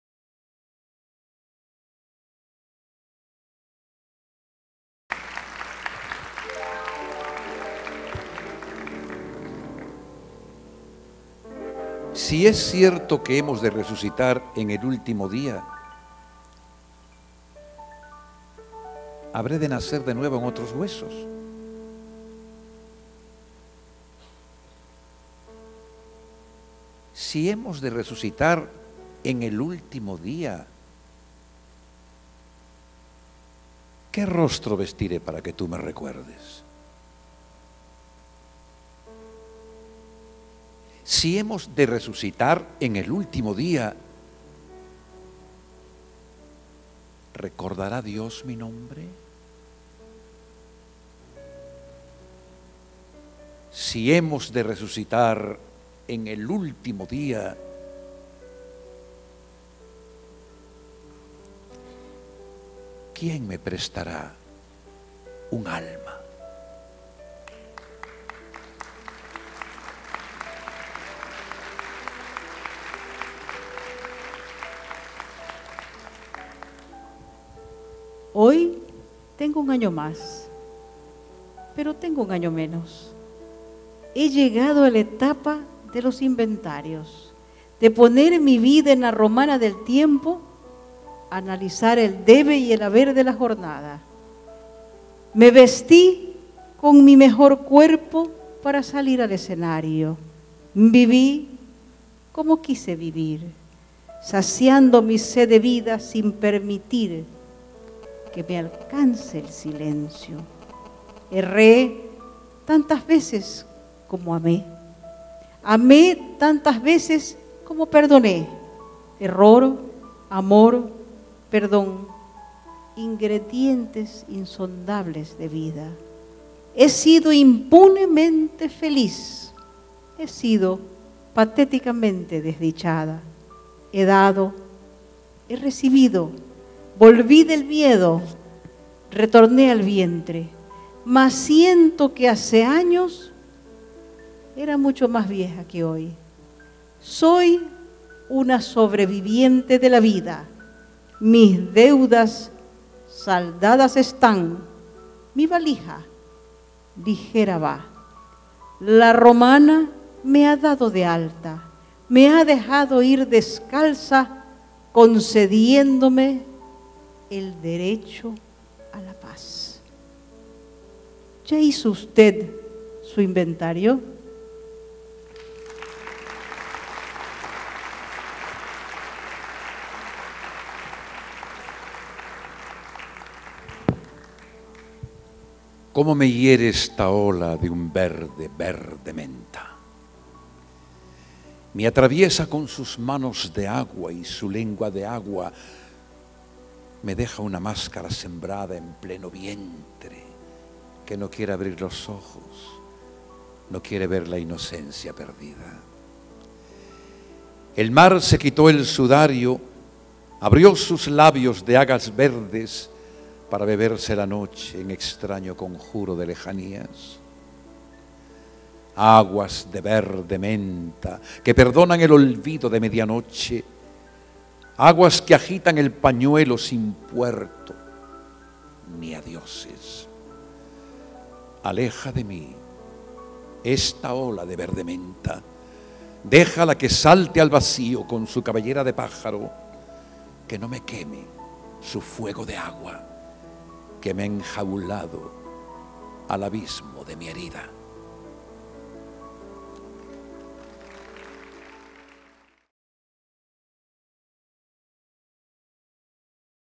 Poesia de Rosa Amelia Alvarado Roca
Poesía de Rosa Amelia Alvarado Roca, acompañada por el actor Antonio Santos Menor. 2012.